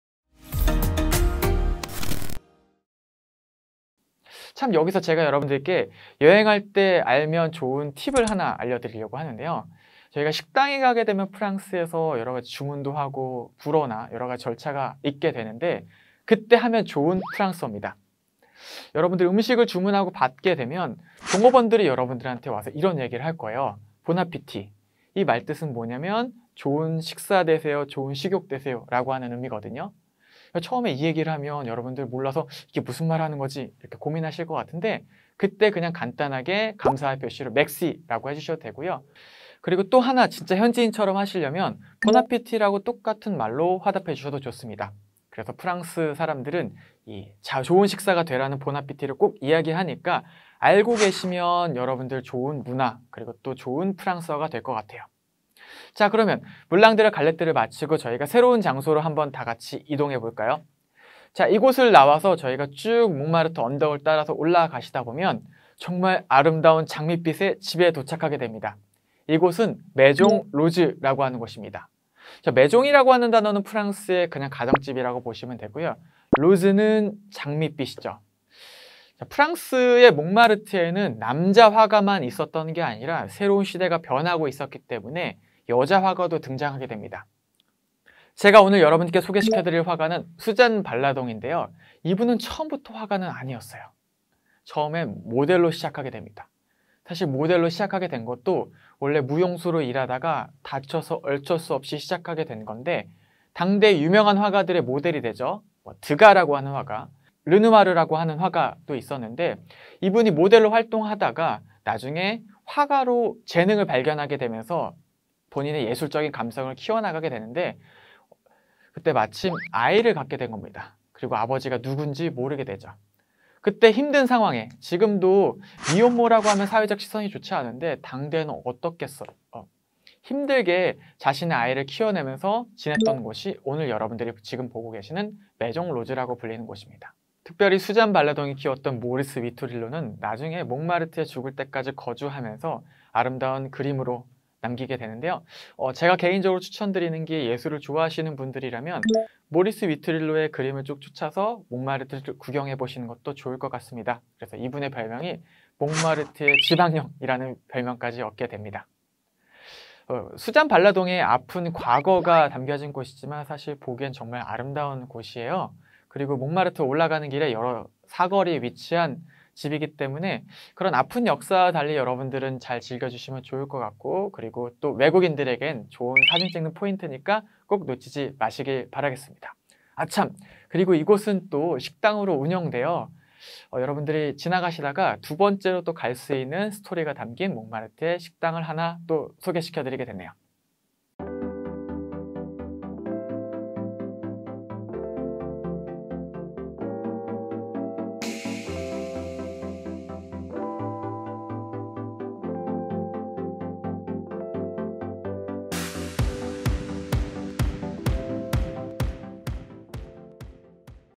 [랜선투어] 몽마르트르 언덕의 핑크하우스, 메종로즈
관광객들의 인기 포토존인 이 곳, 메종로즈는
모델이었던 수잔 발라동이 화가로서 아이를 키우며 어렵게 지냈던 곳입니다.
수잔 발라동의 아픈 과거이지만 아름다운 이 곳,
영상으로 만나볼까요?

랜선투어, 몽마르트르편

▼▼또 다른 폴라리스TV를 만나보고 싶다면▼▼
LG U+ 134번 / KT olleh tv 226번 / SK Btv 249번